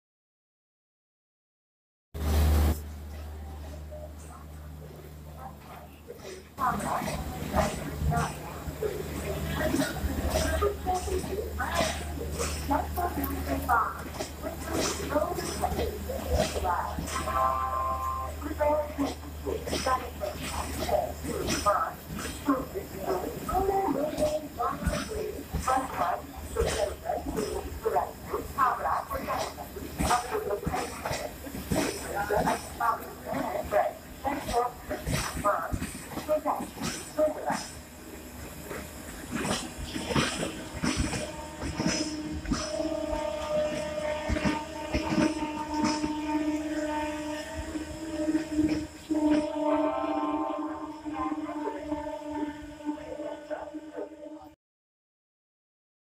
12359 Kolkata - Patna Garib Rath Express skipping Burdwan station
12359 Kolkata - Patna Garib Rath Express skipping Burdwan station